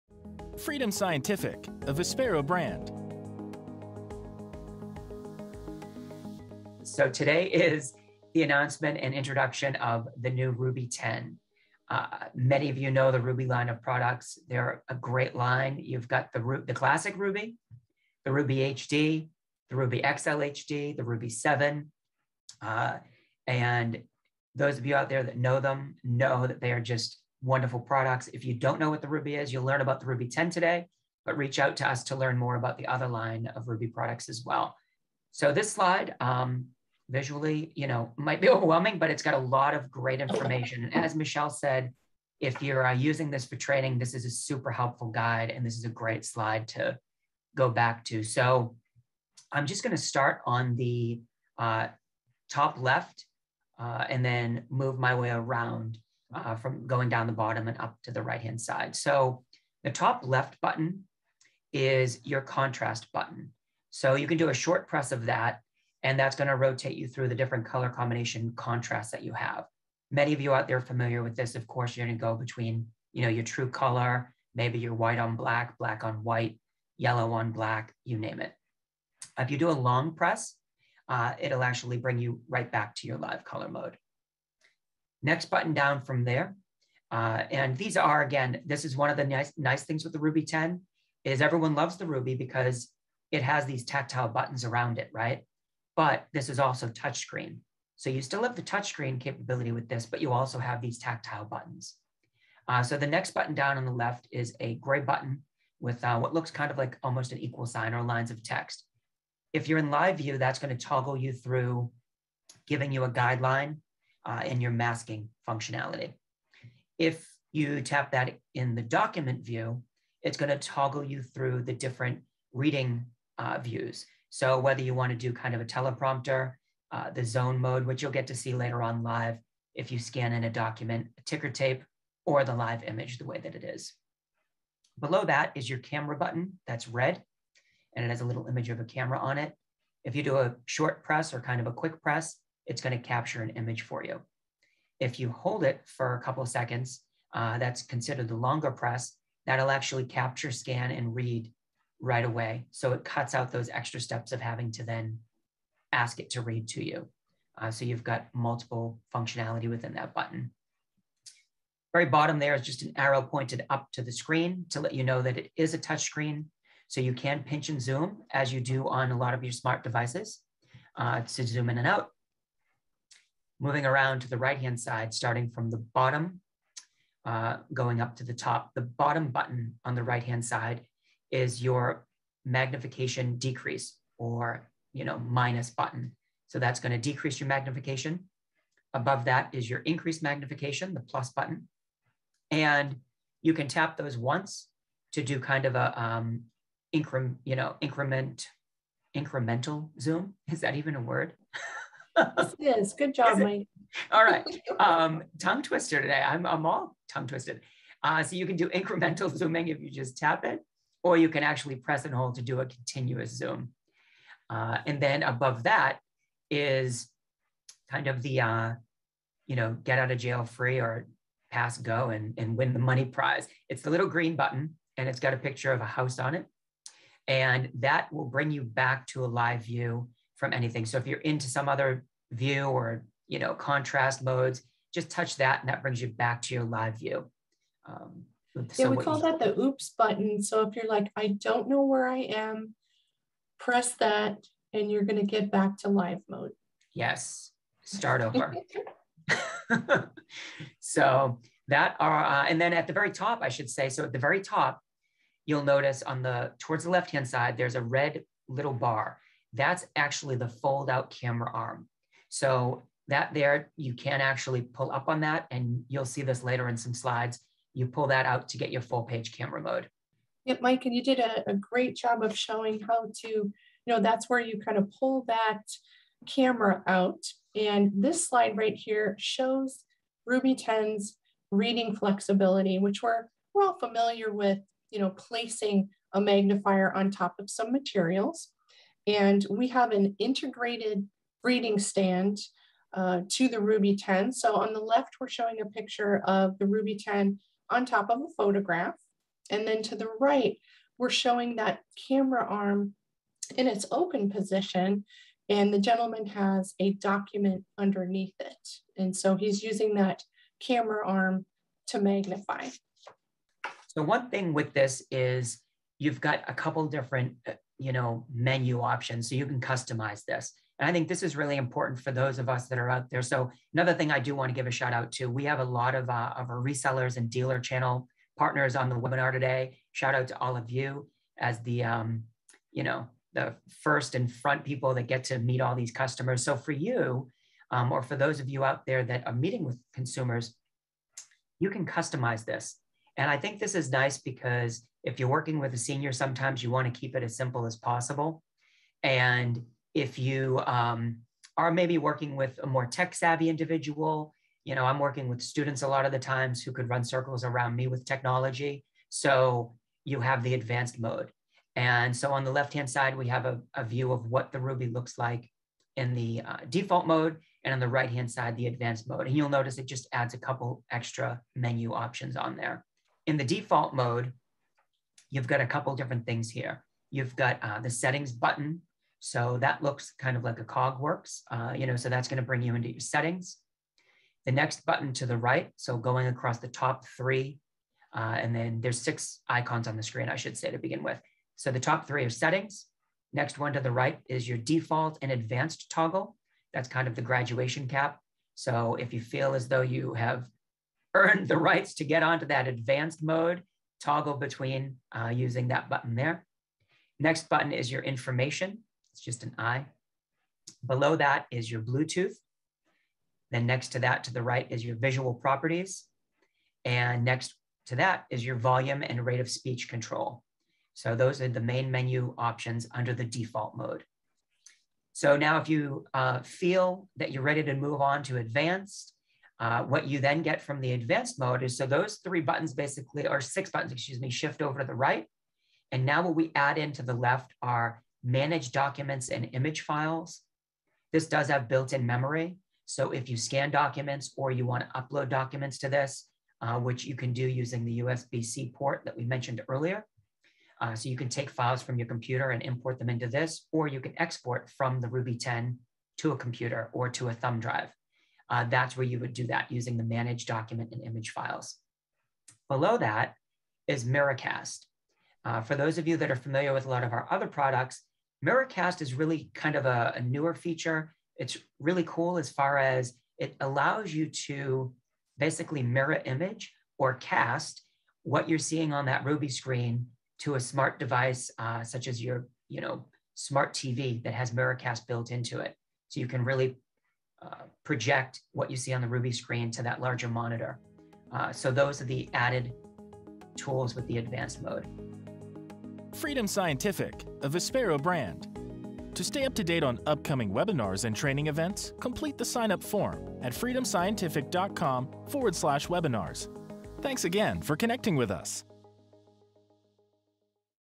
Introducing the all new Ruby 10 Video Magnifier: Overview
Our RUBY video magnifiers have become some of the most popular low vision products on the market today, and we’re so excited to announce the newest addition: the RUBY 10!

The RUBY 10 video magnifier features a large, 10-inch touchscreen display and three dedicated cameras to help you with your everyday tasks. Use the RUBY to magnify magazines, photos, letters, labels, and other fine print.